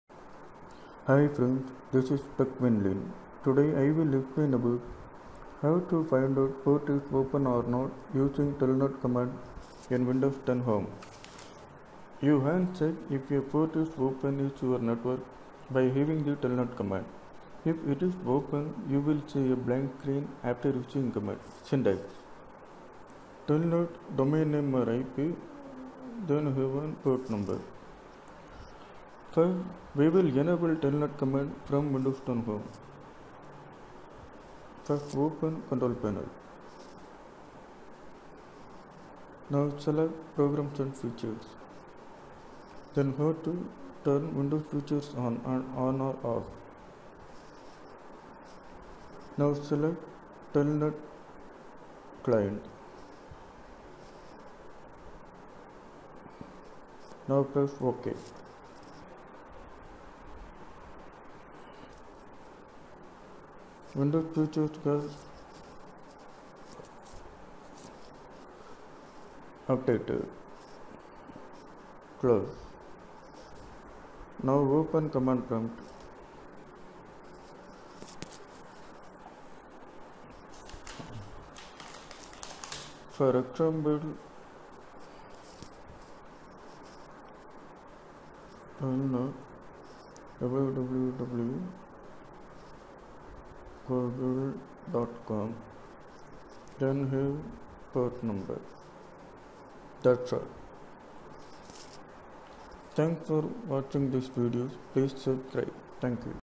How to Check if port is open or not in windows 10 home?
Tech WinLin,The Technical supporter for windows and Linux operating systems.This video describe how to check Check if port is open or not  in windows 10 home.